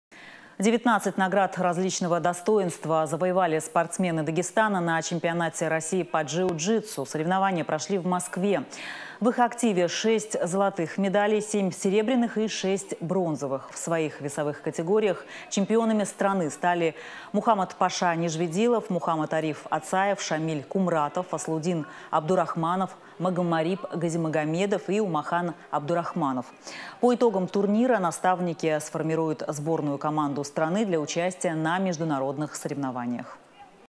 На ЧР по джиу-джитсу дагестанцы завоевали 19 наград
Девятнадцать наград различного достоинства завоевали спортсмены Дагестана на Чемпионате России по джиу-джитсу. Соревнования прошли в Москве. В их активе - шесть золотых медалей, семь серебряных и шесть бронзовых.